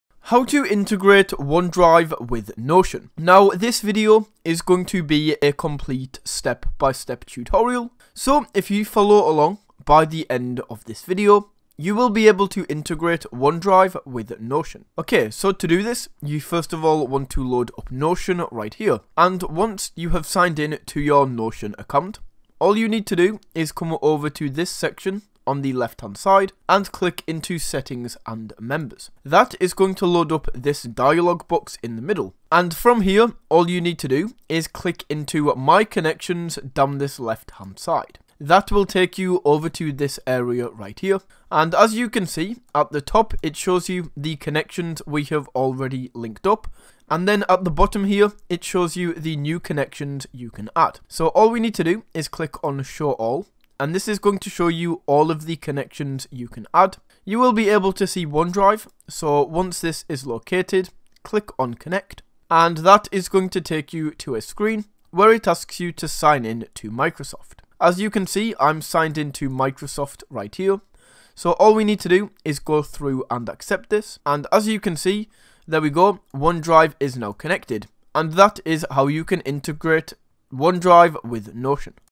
How To Integrate One Drive With Notion
In this tutorial I will show you How To Integrate One Drive With Notion